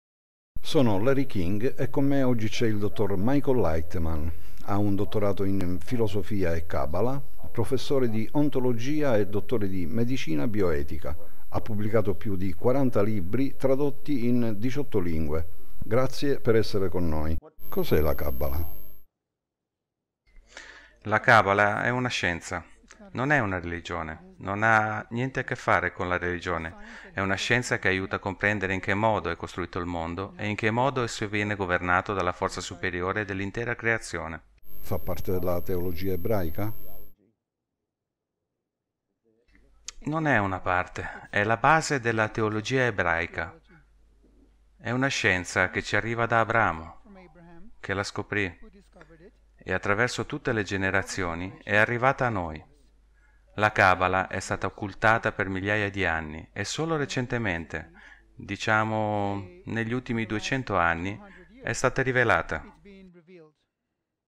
Larry King intervista Michael Laitman - Che cos'è la Kabbalah?
Una parte dell'intervista di Michael Laitman in cui Larry King chiede: Che cos'è la Kabbalah? Fa parte della teologia ebraica?

Larry King: Sono Larry King, con me oggi c’è il Dottor Michael Laitman. Ha un dottorato in Filosofia e Kabbalah, Professore di Ontologia e Dottore di Medicina Bioetica. Ha pubblicato più di 40 libri tradotti in 18 lingue. Grazie per essere con noi. Che cos’è la Kabbalah?

Dr. Laitman: La Kabbalah è una scienza. Non è una religione. Non ha niente a che fare con la religione. È una scienza che aiuta a comprendere in che modo è costruito il mondo e in che modo esso viene governato dalla Forza Superiore della Creazione intera.

L. King: Fa parte della teologia ebraica?

Dr. Laitman: Non è una parte, è la base della teologia ebraica. È una scienza che ci arriva da Abramo, che la scoprì, e attraverso tutte le generazioni, è arrivata a noi.  La Kabbalah è stata occultata per migliaia di anni e solo recentemente, diciamo negli ultimi 200 anni, è stata rivelata.